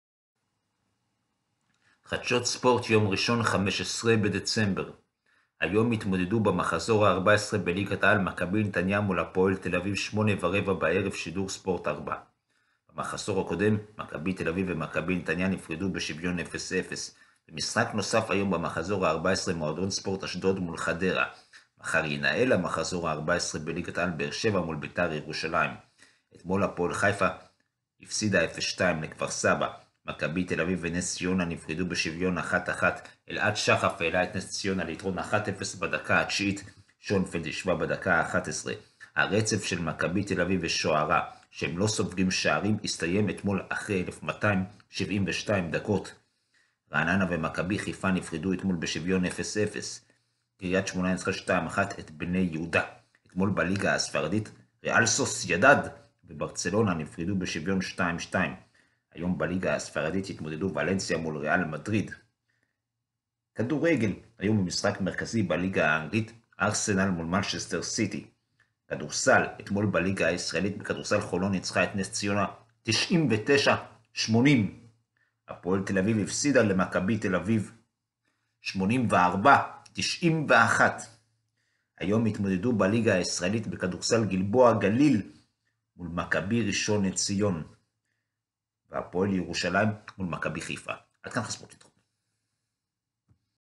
יום 1709 -חדשות ספורט ראשון 15.12.19 - מכבי נתניה מול הפועל ת"א - ליגה
מכבי  נתניה 4 הפועל  ת"א 0  מלמד עם  צמד
מ.ס אשדוד  0 חדרה 2
ולנסיה  1 ריאל  מדריד  1
ארסנל  0 מנצ'סטר  סיטי  3
גלבוע גליל 75 מכבי  ראשון  לציון  96
הפועל ירושלים  95 מכבי חיפה  81